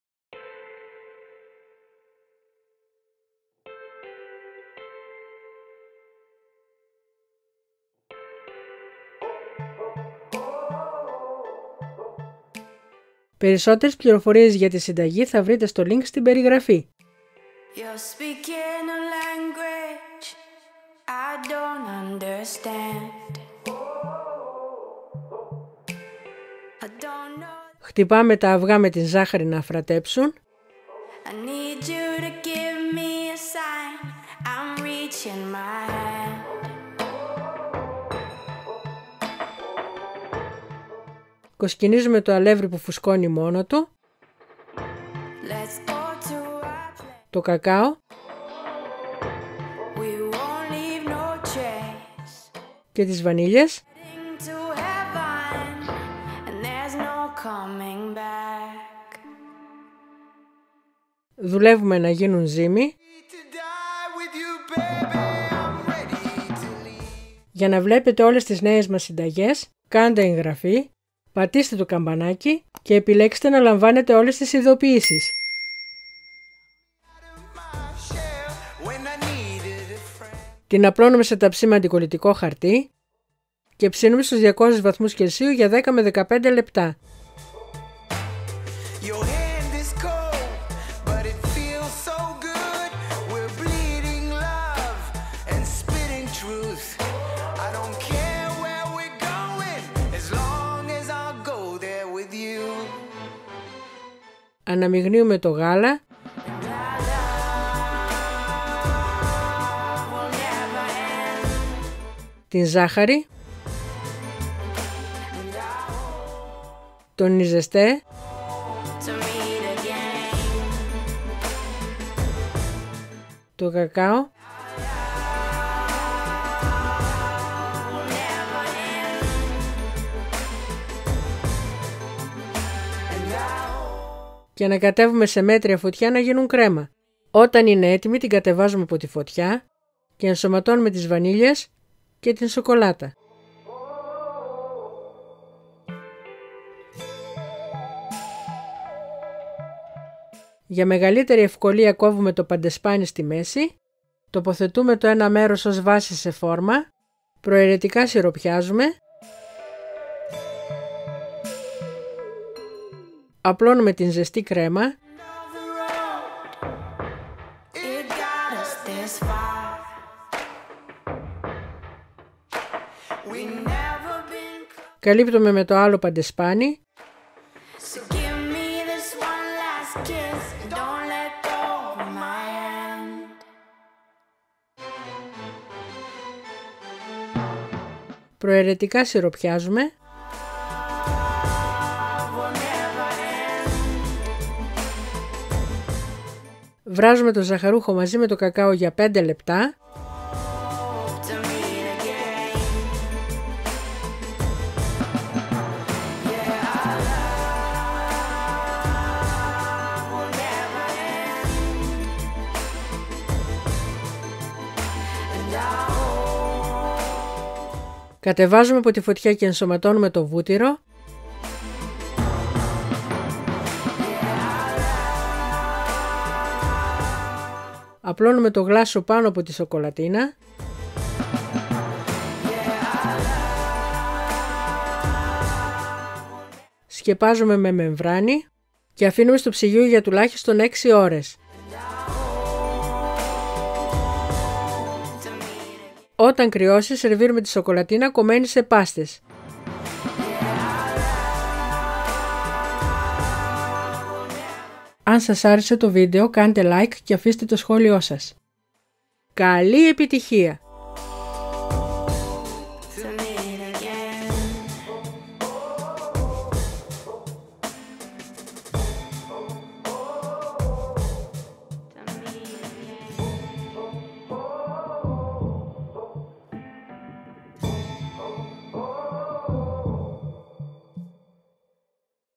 Σοκολατίνα πάστα χωρίς κρέμα γάλακτος | foodaholics
Σοκολατίνα πάστα χωρίς κρέμα γάλακτος | foodaholics. Πάστα μπουκιά και συγχώριο δεν έχει να ζηλέψει τίποτα από τις πάστες ζαχαροπλαστείου. Δοκιμάστε τη και θα το διαπιστώσετε και εσείς. Καλή σας όρεξη.

Συστατικά:

Παντεσπάνι
 165 γρ φαρίνα
 4 αυγά
 200 γρ ζάχαρη
 3 κουταλιές κακάο
 2 βανίλιες
 Κρέμα
 1 λίτρο γάλα
 125 γρ σοκολάτα
 250 γρ ζάχαρη
 50 γρ κακαό
 100 γρ νισεστέ
 2 βανίλιες
 Γλάσο
 1 κουτί ζαχαρούχο
 3 κουταλιές κακάο
 60 γρ βούτυρο

  Sponge cake
  165 g of flour
  4 eggs
  200 g of sugar
  3 tablespoons cocoa
  2 vanillins
  Cream
  1 litre of milk
  125 grams of chocolate
  250 g of sugar
  50 g of cocoa
  100 grams corn flour
  2 vanillins
  Icing
  1 box of sugar
  3 tablespoons cocoa
  60 g butter